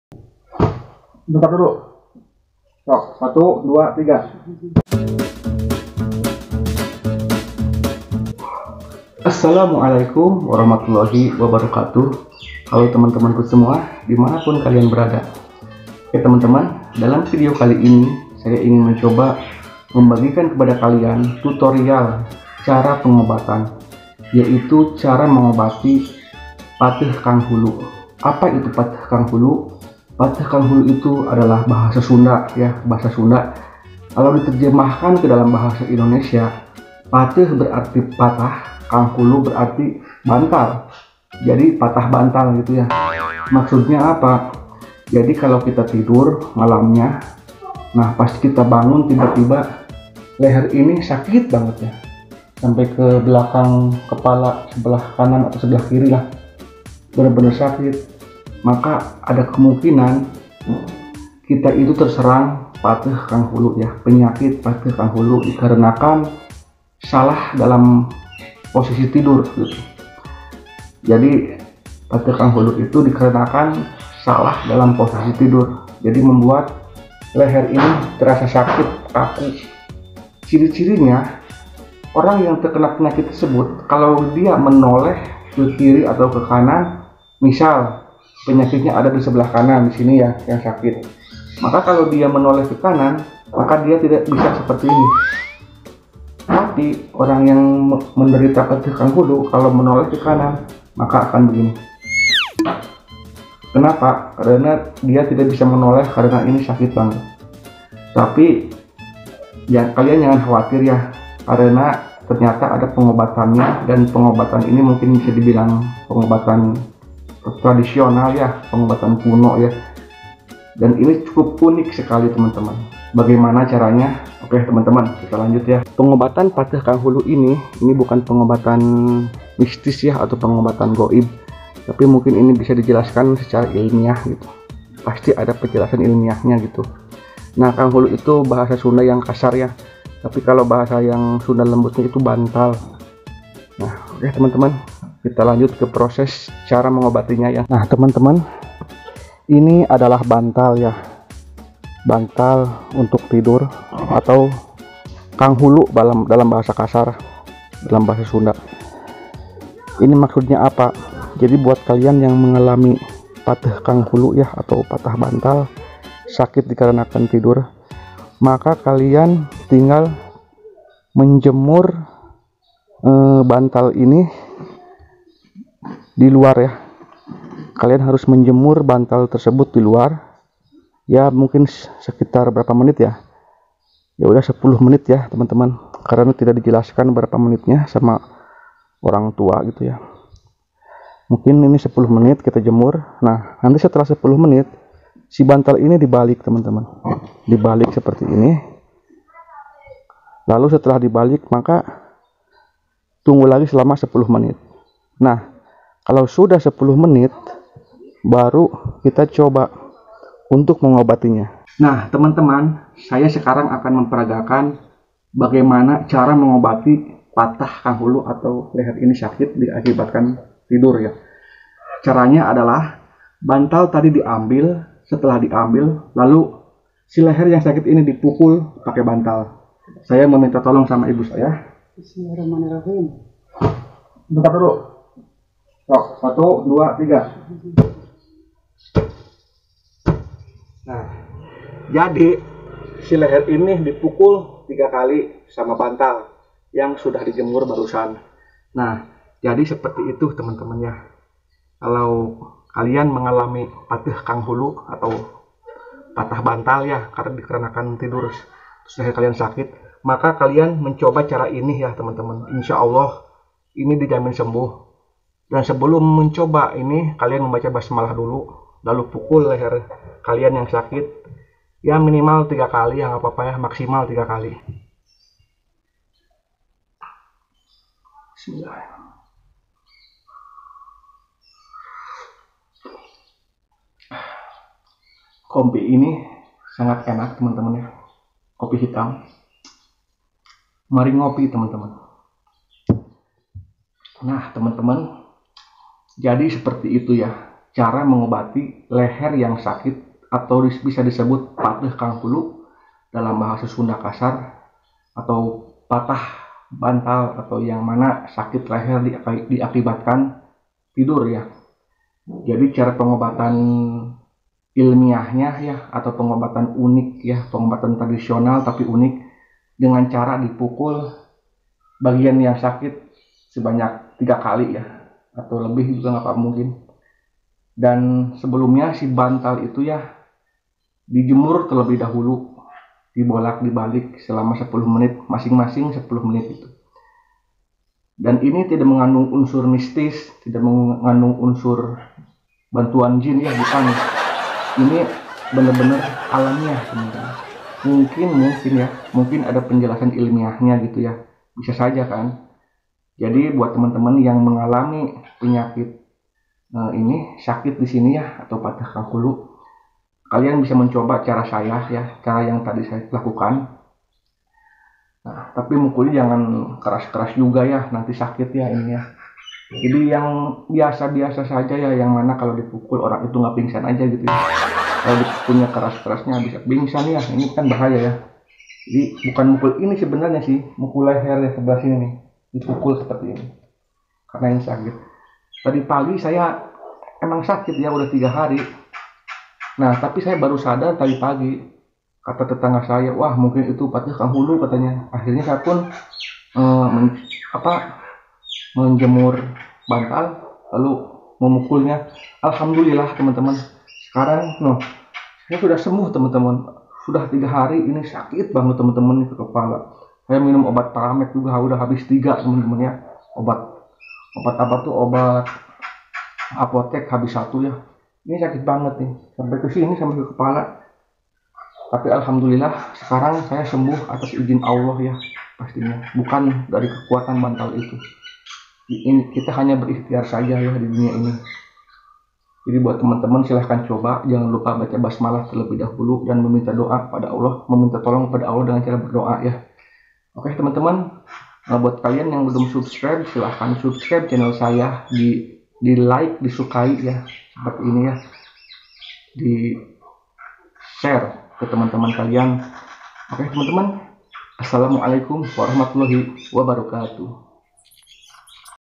Cara Mengobati Sakit Leher Atau Pateh Kanghulu
Penyakit ini akibat salah dalam posisi tidur yang mengakibatkan sakit leher. Akibat lainnya yang terserang penyakit pateh kanghulu atau sakit leher yaitu tidak bisa menoleh karena leher terasa kaku dan sakit. Dan penyakit itu bisa menjalar ke kepala yang menyebabkan sakit kepala. Untuk mengobatinya sangat mudah dan unik. Caranya dengan memukul leher yang sakit dengan bantal yang sudah di jemur terlebih dahulu.